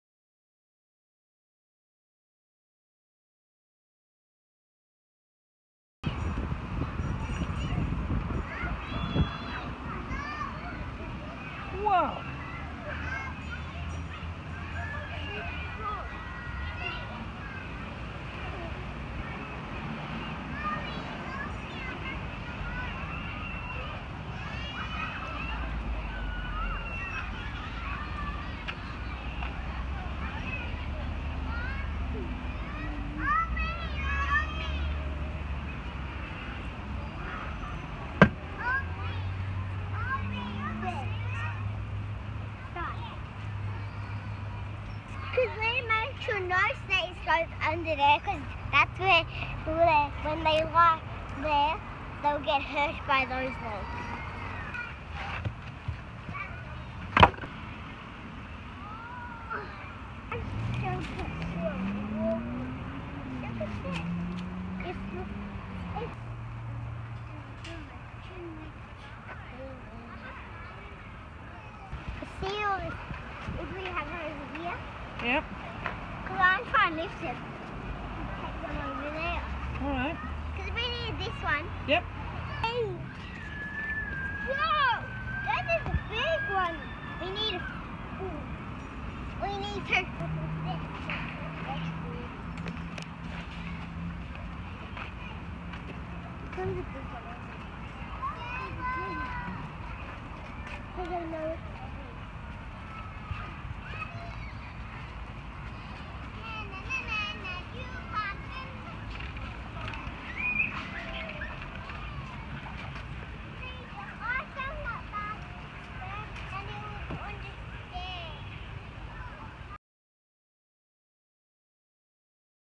EYLF 4.4 Children resource own learning
Loose parts rocks and logs on our top yard allow children to move heavy logs, use communication, teamwork and divergent thinking. In this case they are creating an unpleasant environment for snakes using logs.